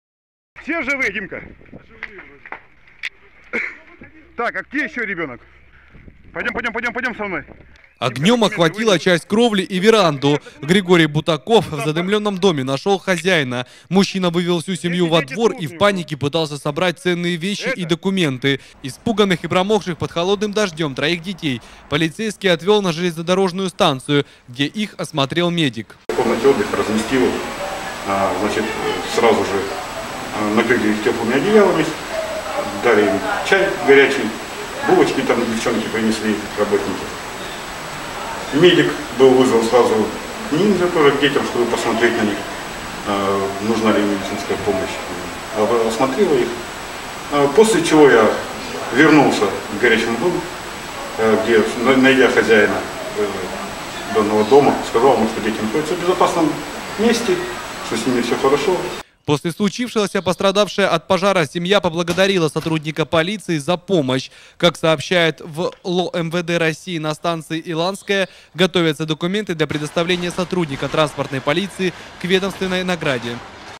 ПОМОГ ПРИ ПОЖАРЕ
Сотрудник линейного отдела эвакуировал детей из ночного пожара. Все произошло в Канске вблизи железнодорожного вокзала. Полицейский находился на маршруте патрулирования, когда увидел зарево над одним из частных домов. Не задумываясь, он бросился туда.